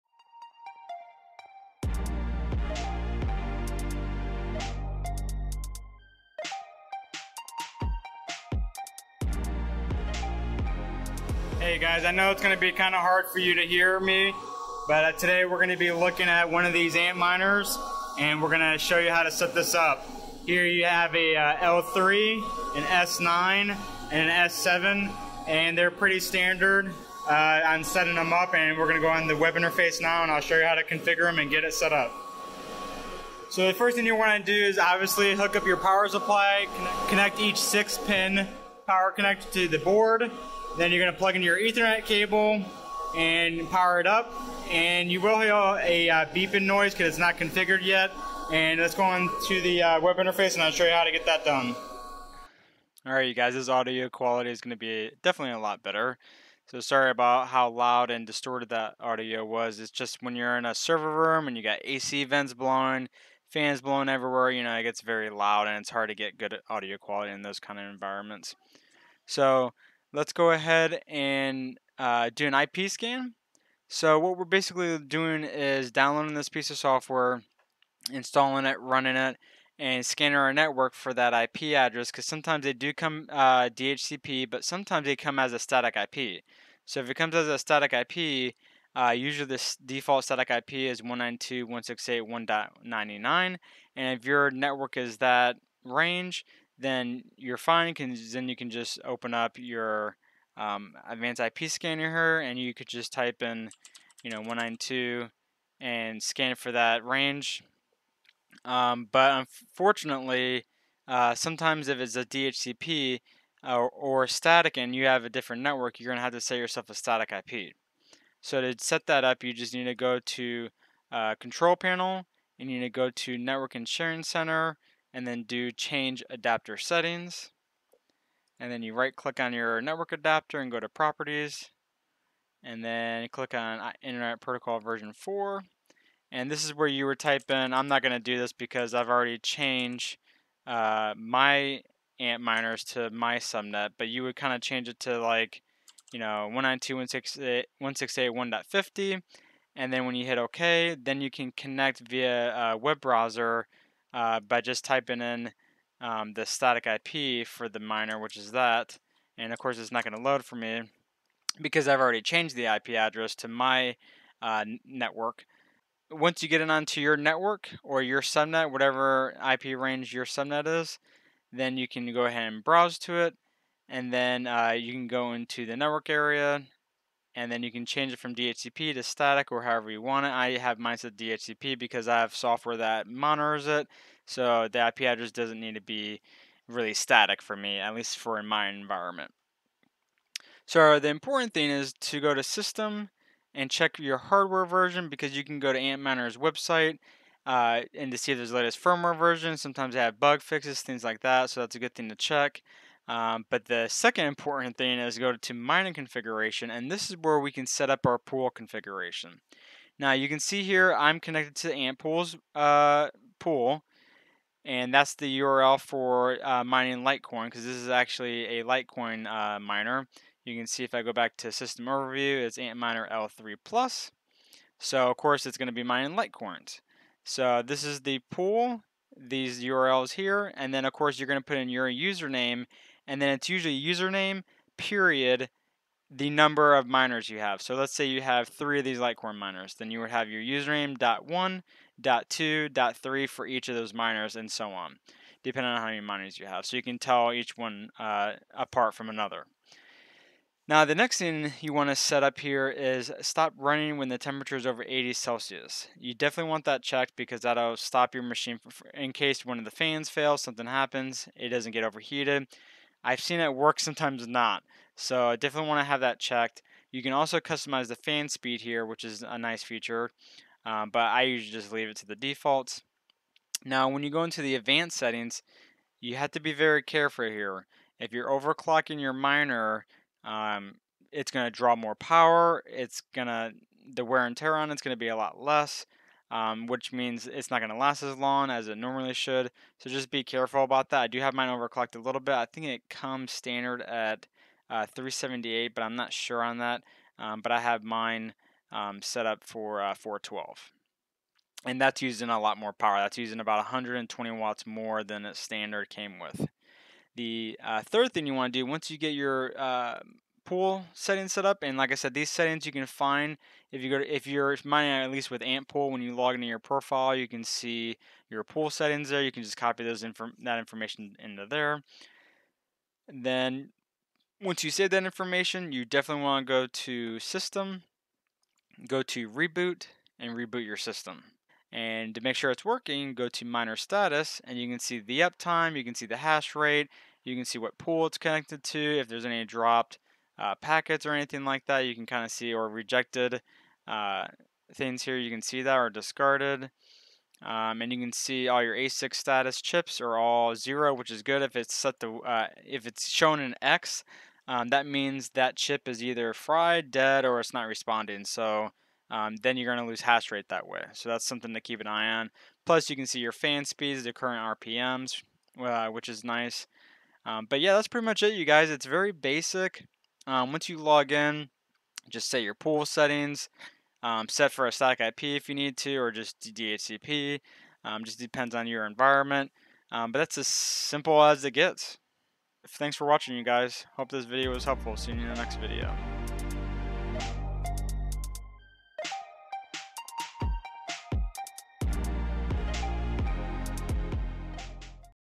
How to setup a Antminer Miner and connect it to a pool to mine
In this video I will show you how to find your antminer on your network using a network scan tool then we will setup the pool information, reboot then see it starting mining.

Scan network with this to find your miner.